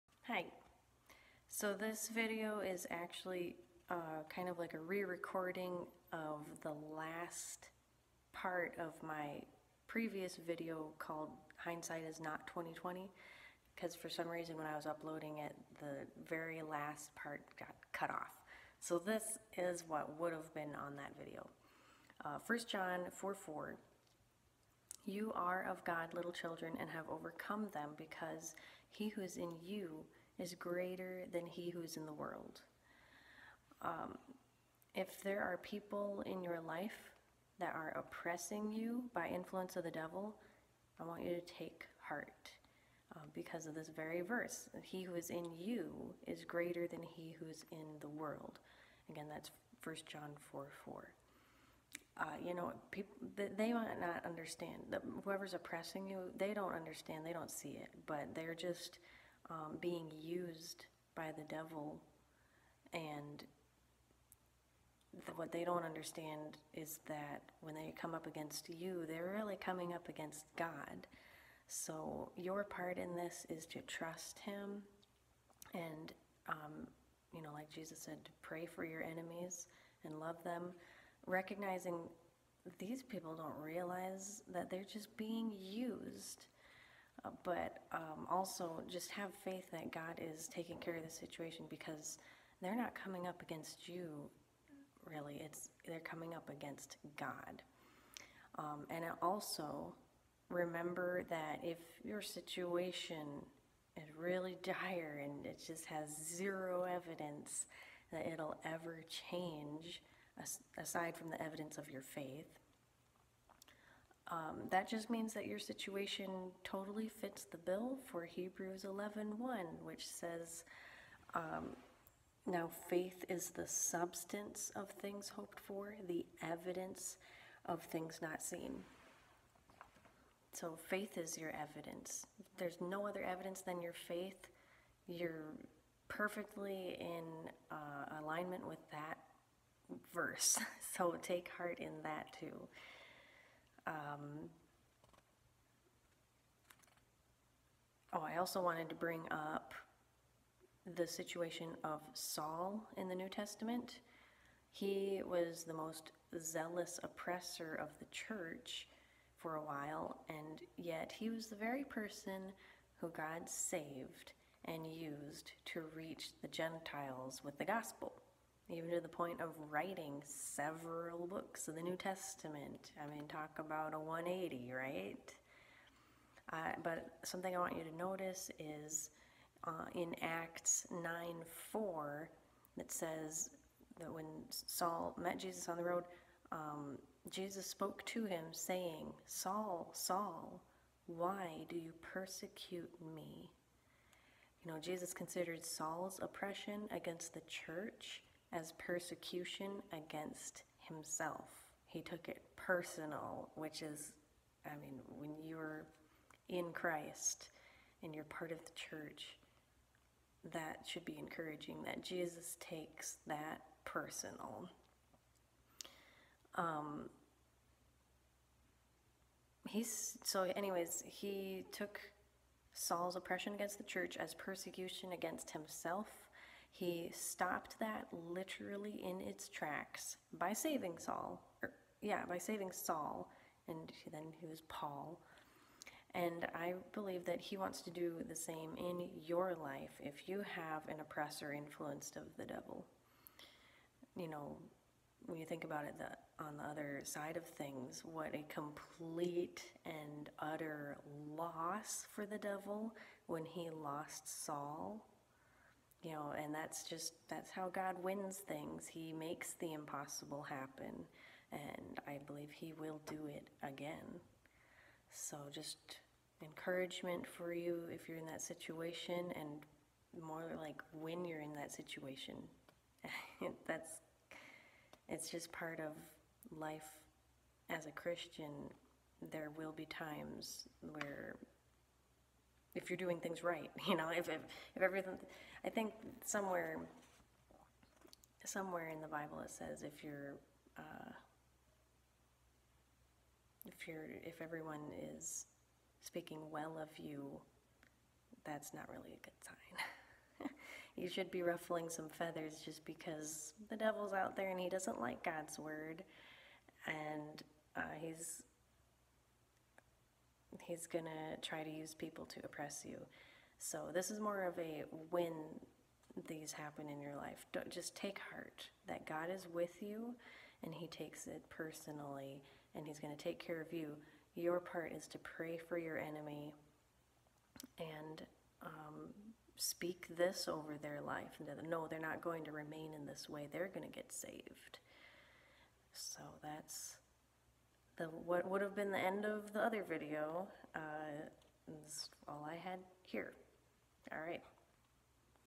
Re-Upload Of The Ending Of "Hindsight ISN'T 20/20"
Here is what would have been said in my previous video titled "Hindsight ISN'T 20/20 This is what it is..." because the end got skipped during the original uploading process.